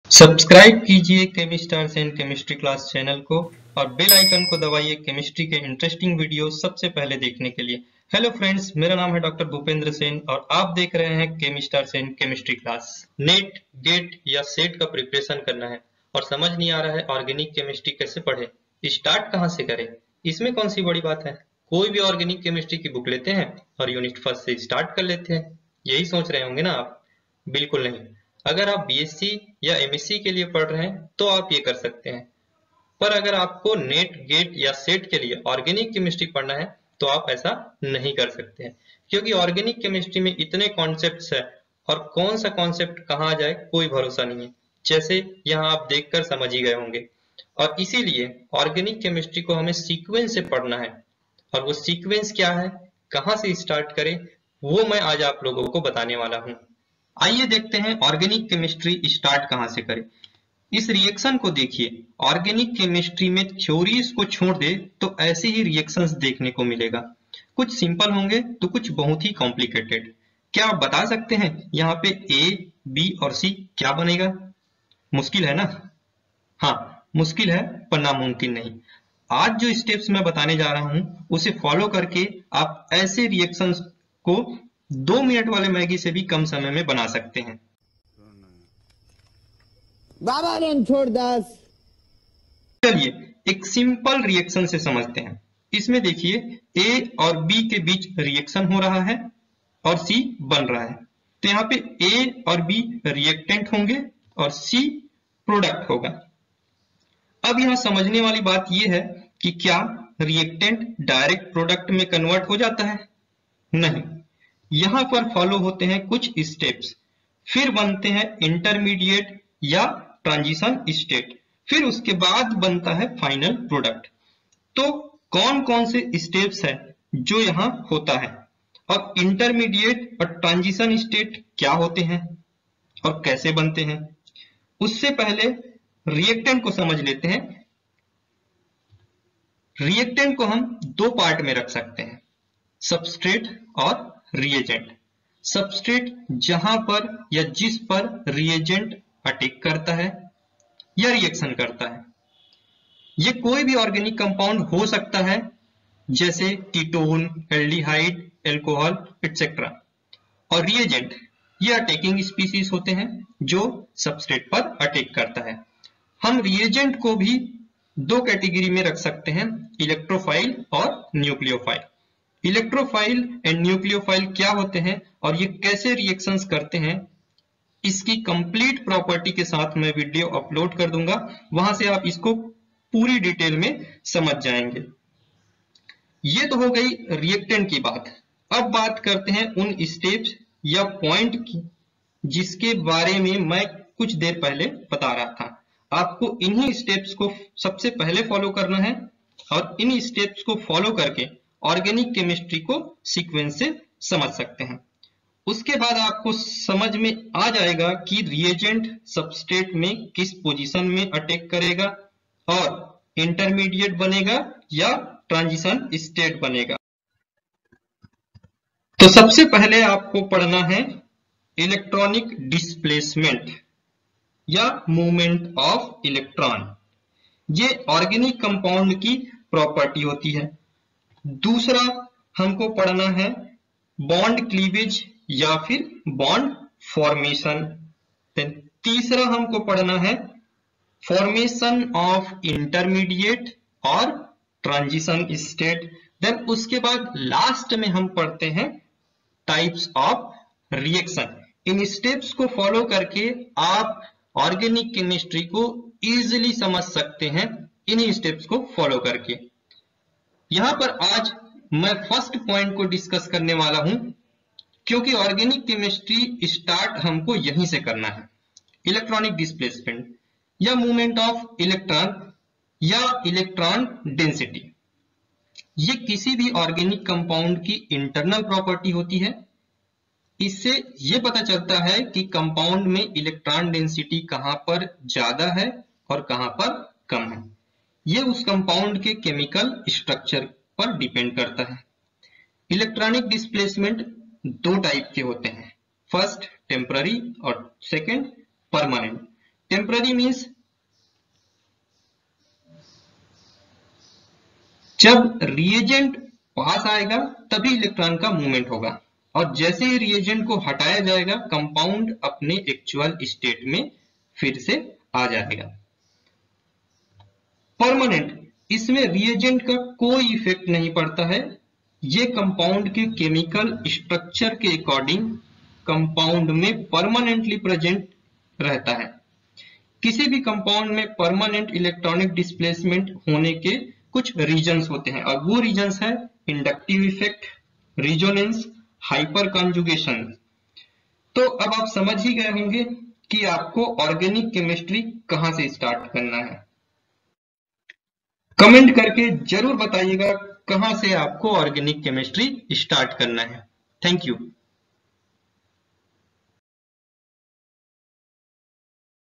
Organic Chemistry Part 1: Organic Chemistry START कहाँ से करूँ…?
Learn where to start Organic Chemistry and what are the steps to follow? Reactant, Substrate, Reagent, Product, Nucleophile, Electrophile, Electronic Displacement, Intermediate, Transition State, Bond Cleavage, Types of Reaction, Reaction Mechanism, Electromeric effect, Inductive effect, Resonance, Hyperconjugation